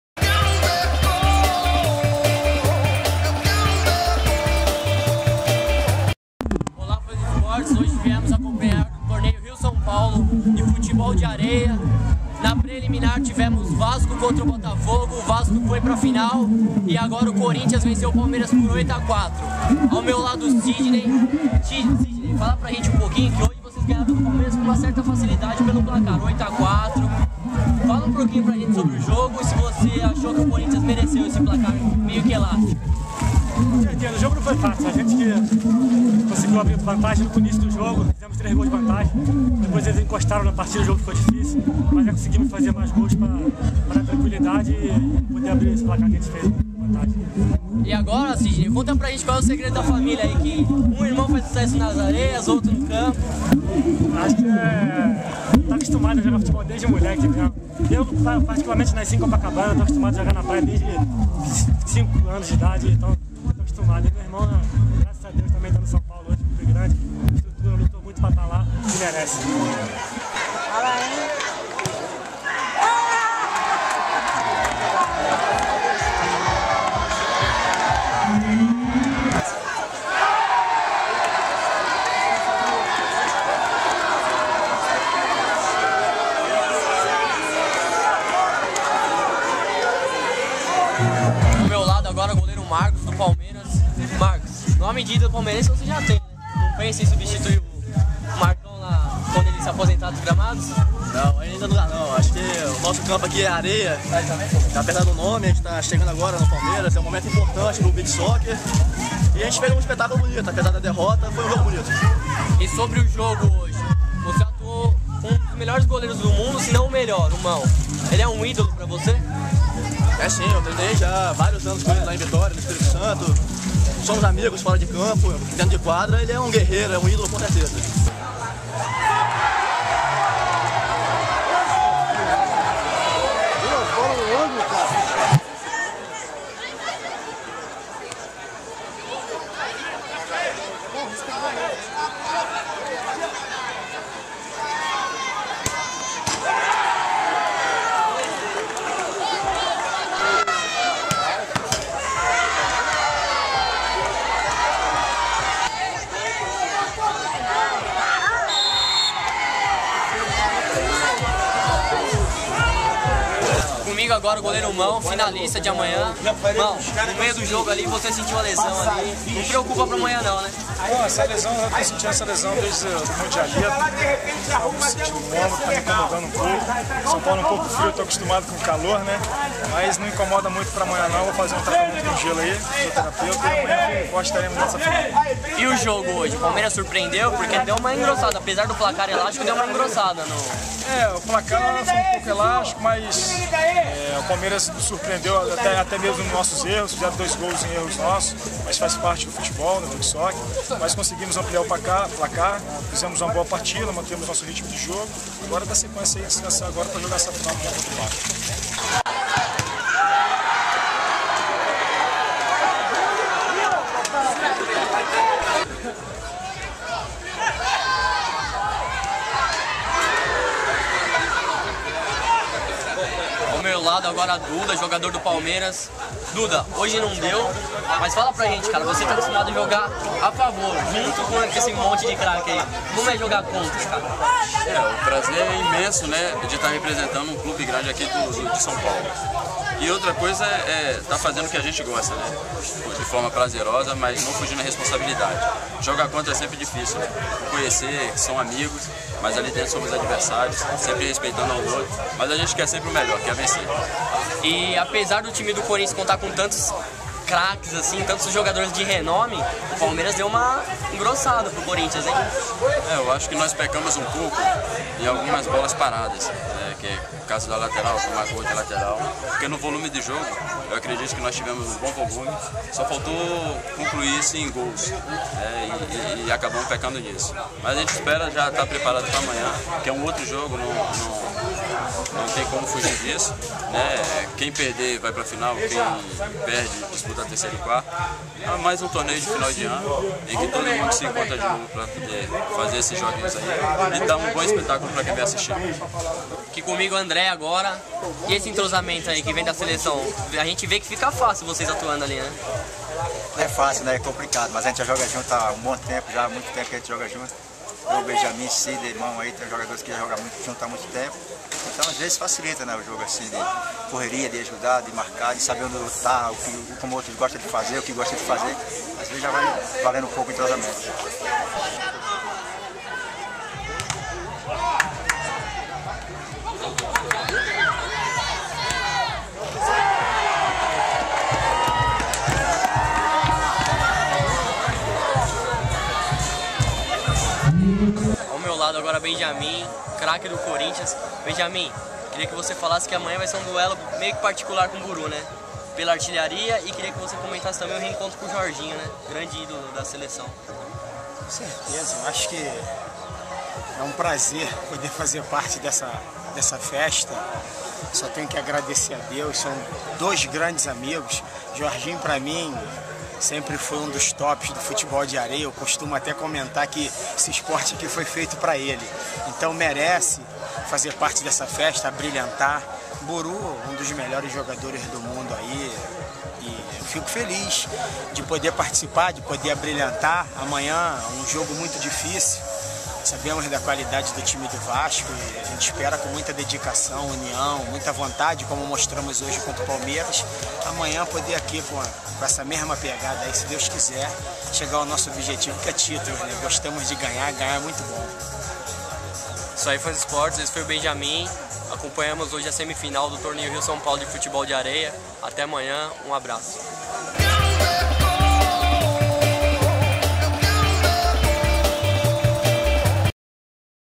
Corinthians 8x4 Palmeiras Beach Soccer no Pelézão 18/12
Os repórteres Dhiego Vicário e Rodrigo Hoschett cobriram a semifinal do Torneio Rio x SP. O Corinthians venceu o Palmeiras por 8 a 4. A partida aconteceu no clube Pelézão, em São Paulo. Veja a matéria completa no